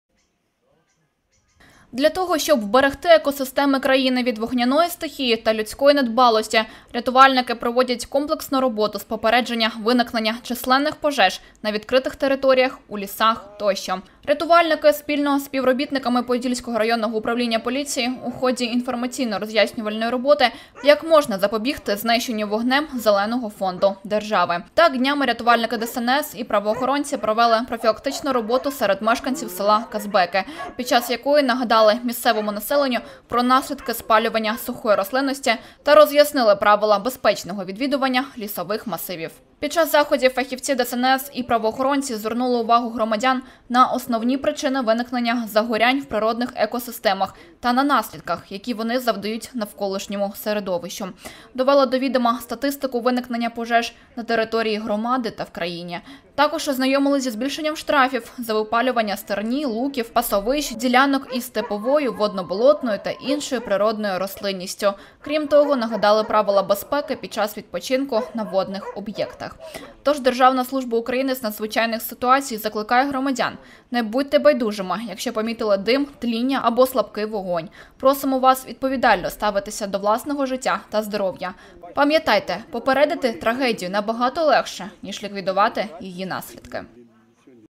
Правоохоронці спільно з рятувальниками провели профілактичний рейд у селі Казбеки
Рятувальники спільно з поліцейськими провели профілактичний рейд, щодо запобігання пожеж у екосистемах в селі Казбеки. Подробиці - у сюжеті.